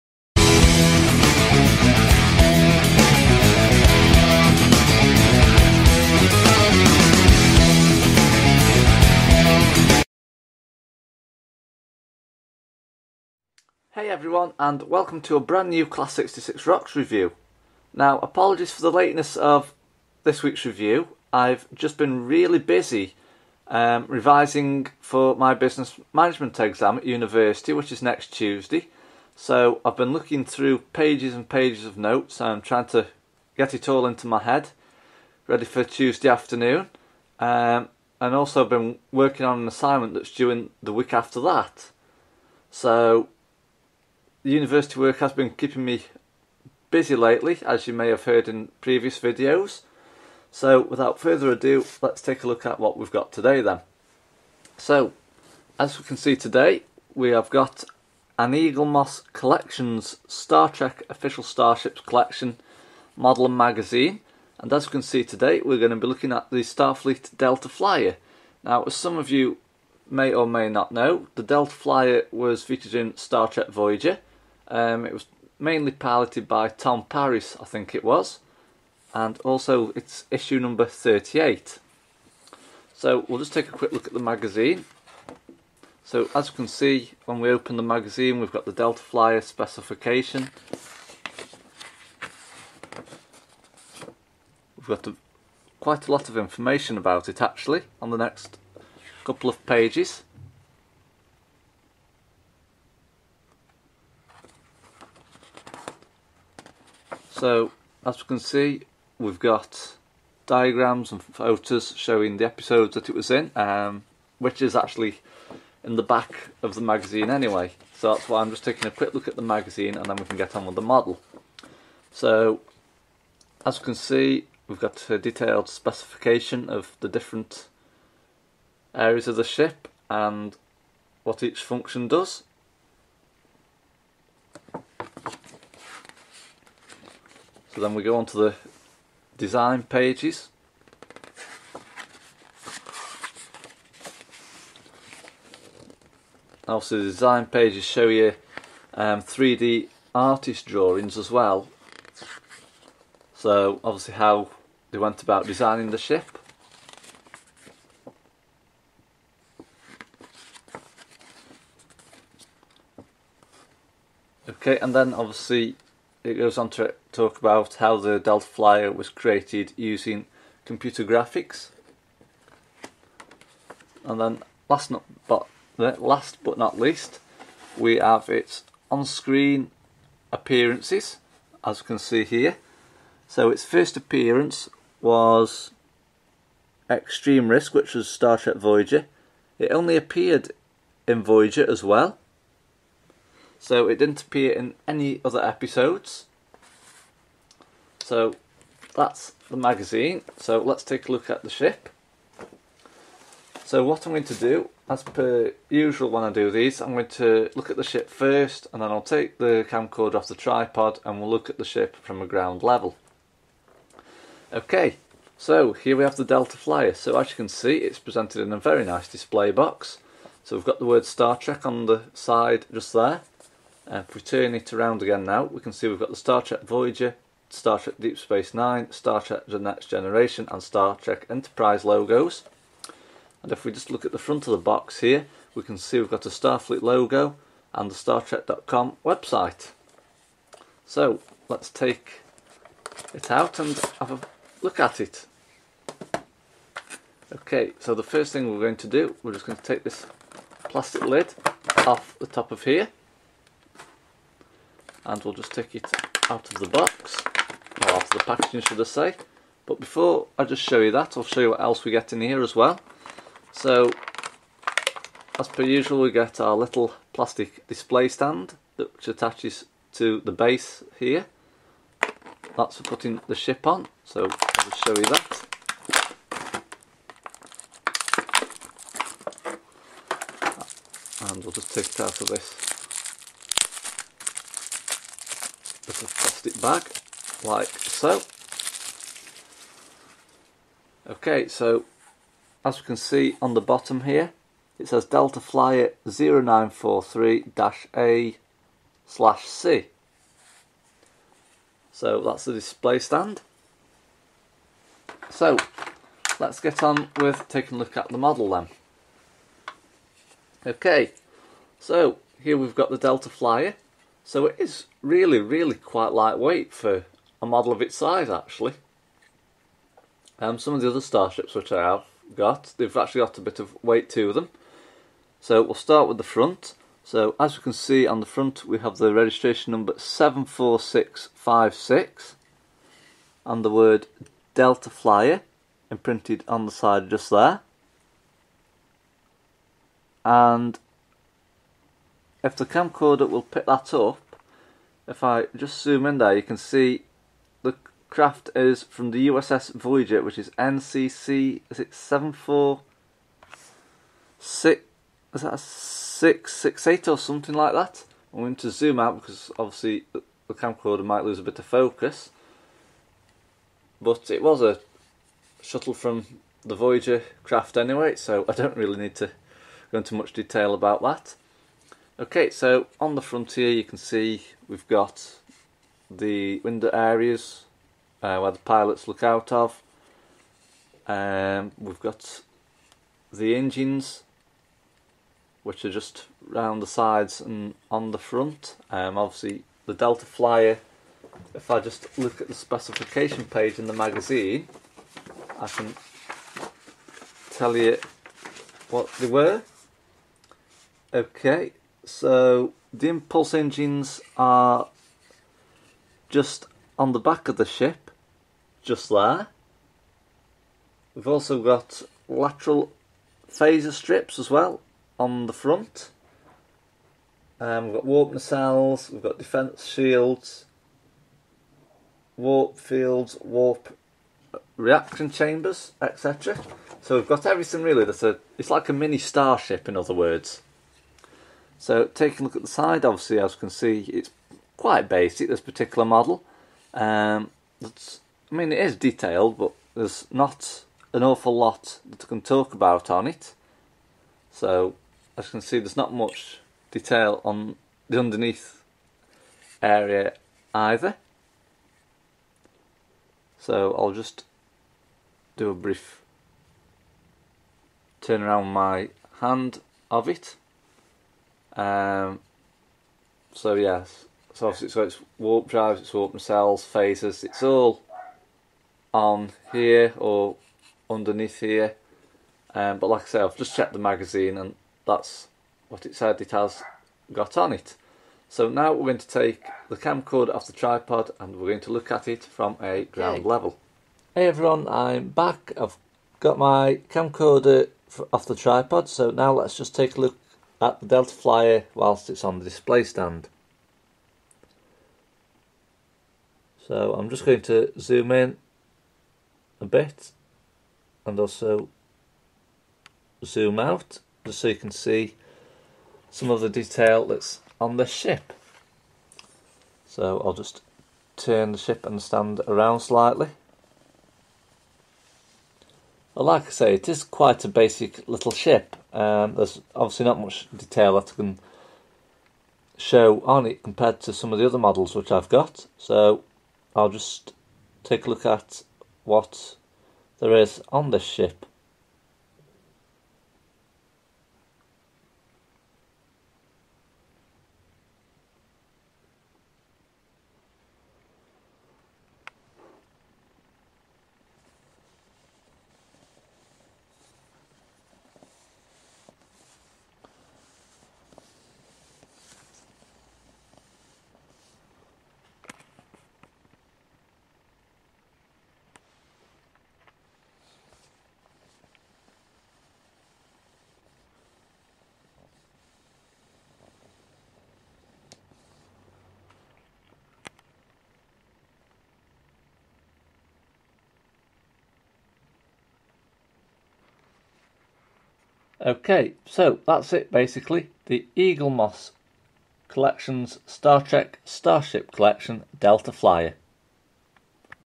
Opening The Eaglemoss Star Trek Collection Starfleet Delta Flyer
Hey everyone, in this video, I take a look at the Eaglemoss Publications Star Trek starship collection. Each issue comes with an authentic replica model, as well as a magazine packed full of information. The model which I am taking a look at in this video, is the Starfleet 74656 Delta Flyer. These models come in a starfleet presentation box, which also contains a display stand